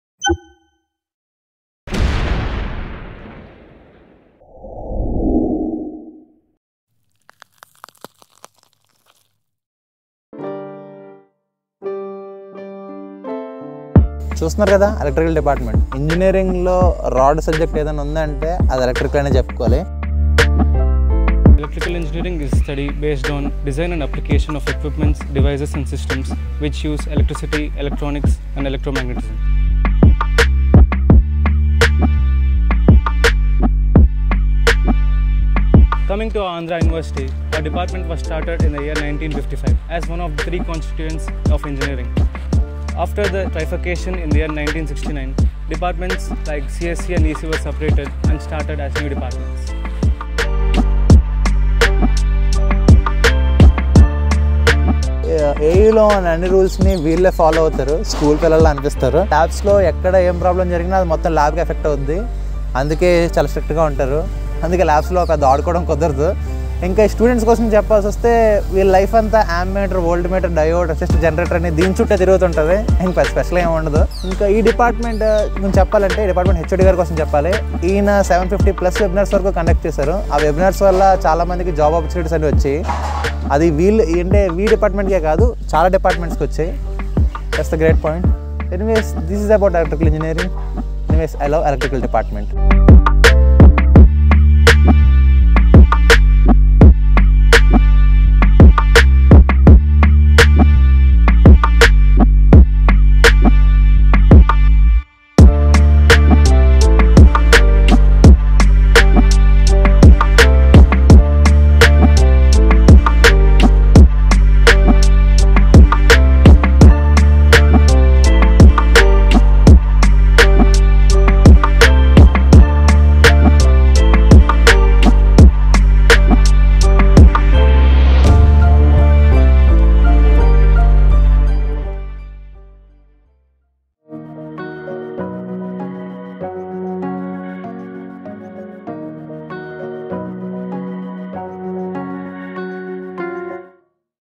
ELECTRICAL ENGINEERING | REASONS WHY I LOVE MY ANDHRA UNIVERSITY| 2017-21 MEMORIAL| CRAZII NIDESH
ANDARI UNIVERSITY PART 2
THIS VIDEO IS A GIFT FROM CRAZII NIDESH TO ANDHRA UNIVERSITY AND HIS FRIENDS , SENIORS & JUNIORS WHO MADE HIS COLLEGE LIFE MUCH MORE CRAZII. This video is about how a AU student enjoyed his most part in his college with his friends
IN THIS VIDEO SPEAKER DESCRIBED ABOUT CHEMICAL DEPARTMENT OF ANDHRA UNIVERSITY IN A FUN AND ATTRACTIVE MANNER

THE CONTENT IN THIS VIDEO IS Quite Interesting and Worth Watching

THIS IS ANDHRA  UNIVERSITY FROM MY SIDE
ANYWAYS & ALWAYS I LOVE ANDHRA UNIVERSITY

A Fun Generator
AU Lover

00:00 - DISCLAIMER
00:36 - VIDEO ENTRY
03:05 - AU HISTORY
04:50 - CHEMICAL ENGINEERING
08:47 - INSTRUMENTATION
12:29 - CIVIL
15:54 - GEO INFOMATICS
19:20 - ARCHITECTURE
22:44 - ELECTRICAL
24:57 - ECE
28:21 - CSE
32:31 - MECHANICAL
35:52 - MARINE
40:17 - METALLURGY
46:35 - CLIMAX
48:46 - END CREDITS

drone shots are used from the video